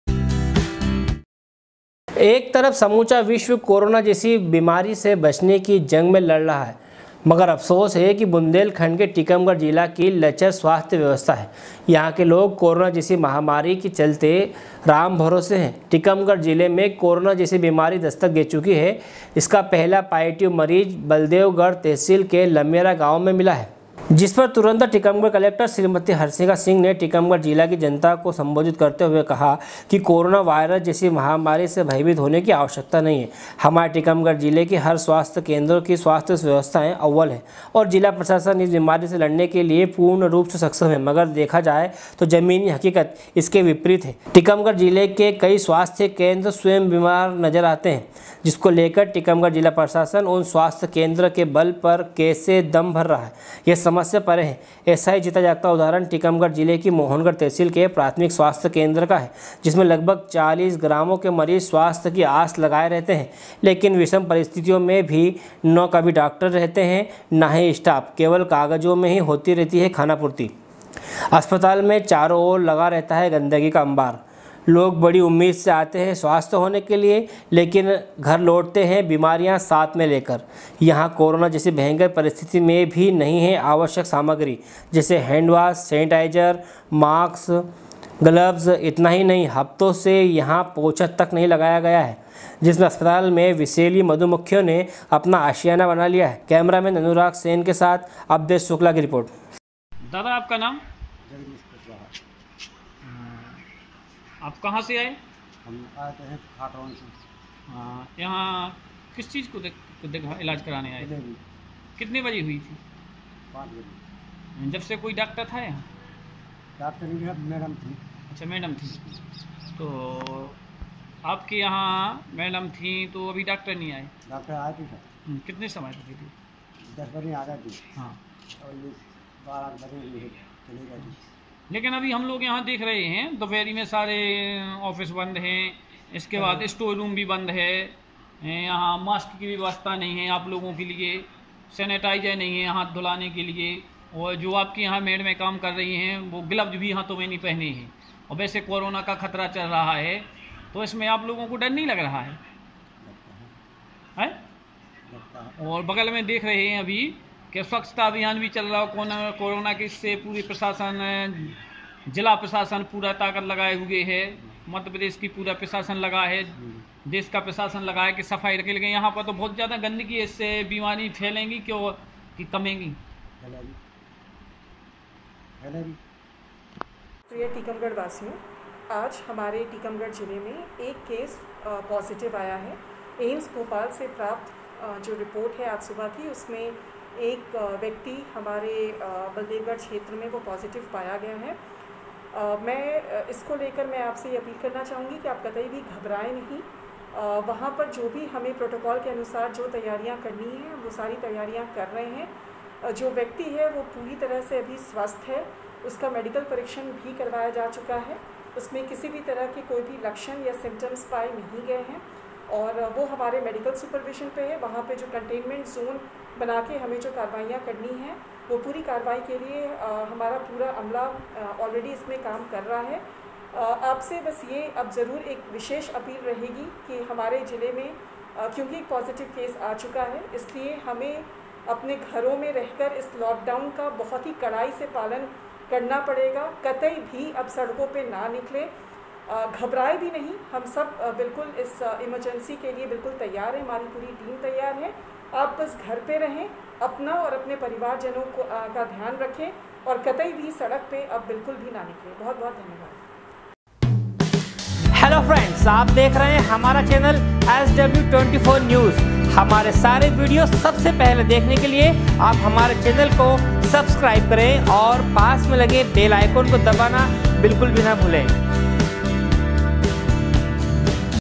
SW 24 News Madhyapradesh
एक तरफ समूचा विश्व कोरोना जैसी बीमारी से बचने की जंग लड़ रहा है मगर अफसोस है कि बुंदेलखंड के टीकमगढ़ जिला की लचर स्वास्थ्य व्यवस्था है यहां के लोग कोरोना जैसी महामारी के चलते राम भरोसे हैं टीकमगढ़ जिले में भी कोरोना जैसी बीमारी दस्तक दे चुकी है इसका पहला पॉजिटिव मरीज बल्देवगढ़ तहसील के लमेंरा ग्राम में मिला है
कैमरा मेन अनुराग सेन के साथ अबधेश शुक्ला की